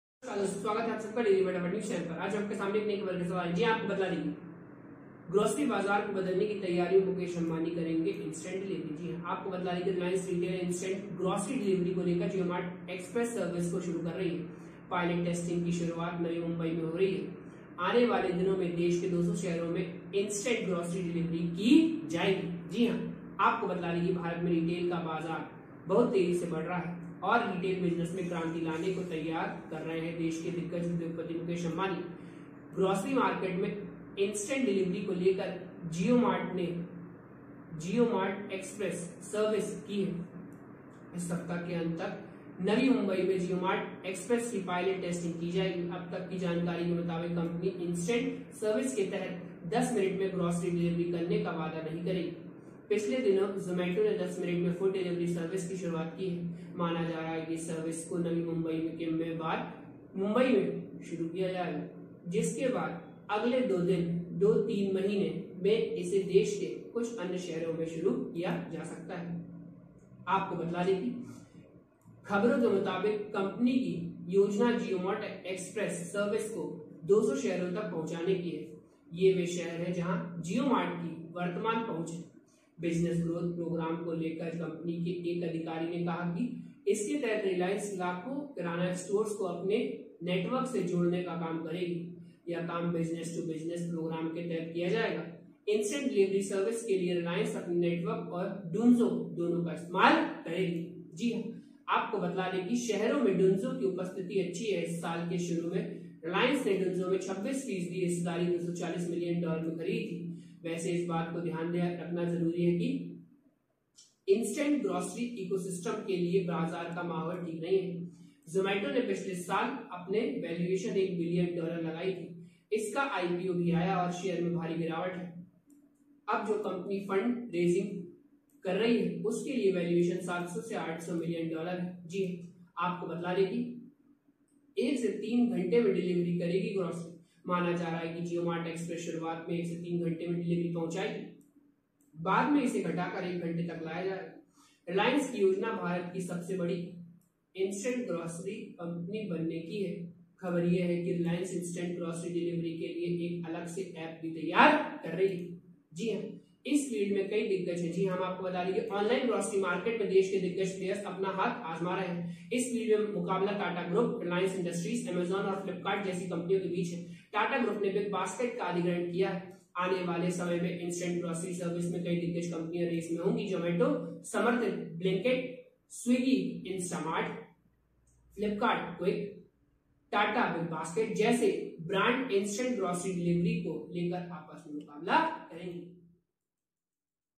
Jio Mart will focus on Instant Delivery | Jio Mart Express | Mukesh Ambani | Reliance
Hi Team,

मुकेश अंबानी तैयारी कर रहे हैं ग्रॉसरी बाजार
 को बदलने की, इंस्टेंट डिलीवरी की तलाश में

Jio Mart Will now Focus on Instant Delivery